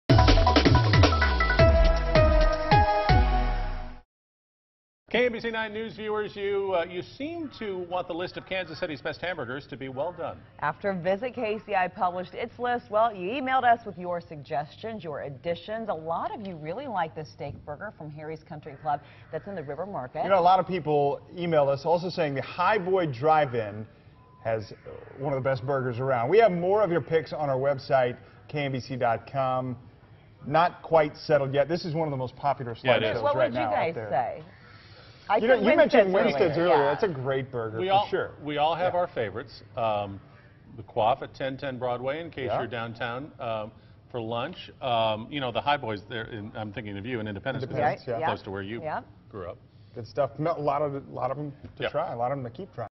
Best burgers in KC
KMBC 9 News' morning show anchors are even talking about some of the places on the list of the best burgers in KC.